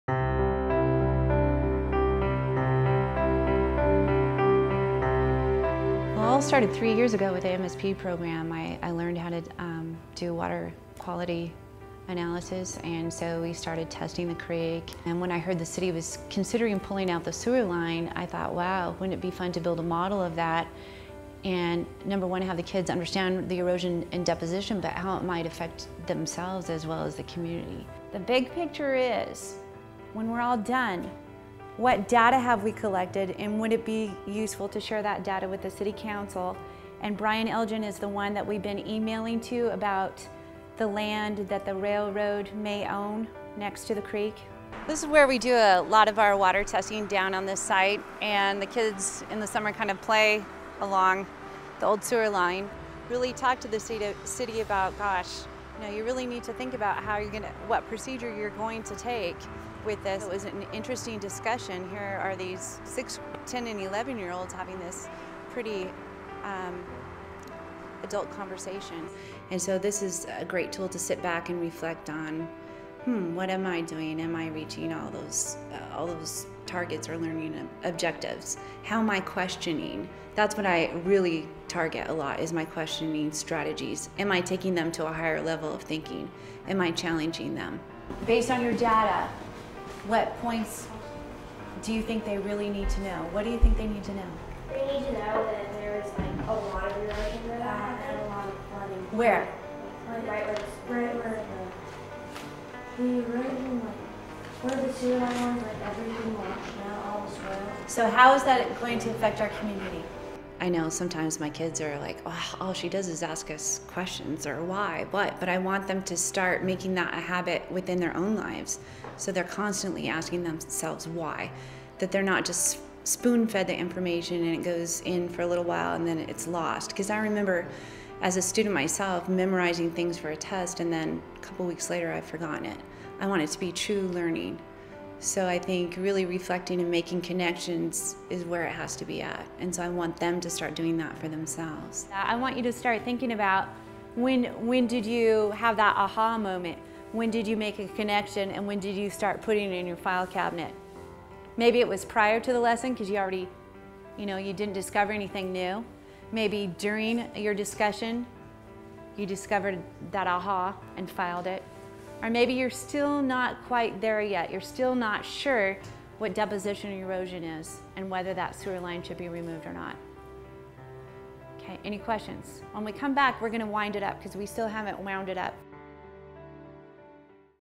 One Teacher's Journey - Powerful Teaching and Learning (Part 2)
While evaluating the Washington State Math Science Partnership, The BERC Group discovered an intriguing story about a 6th Grade teacher in Tekoa, WA who improved her practice using Powerful Teaching and Learning and the STAR Protocol.  Part 2 of 3.

to find out more!